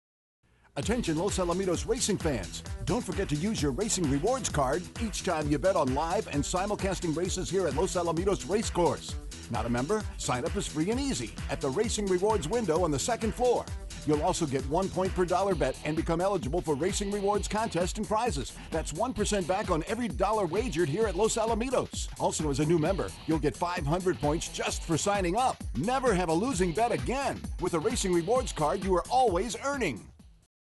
Attention Night Racing Fans! Sign up to the Los Alamitos Racing Rewards program!
On-track racing fans at night, don't forget to sign up to the Los Alamitos Racing Rewards program. It's free, it's fun and you get great rewards when you use your card during the night time racing programs!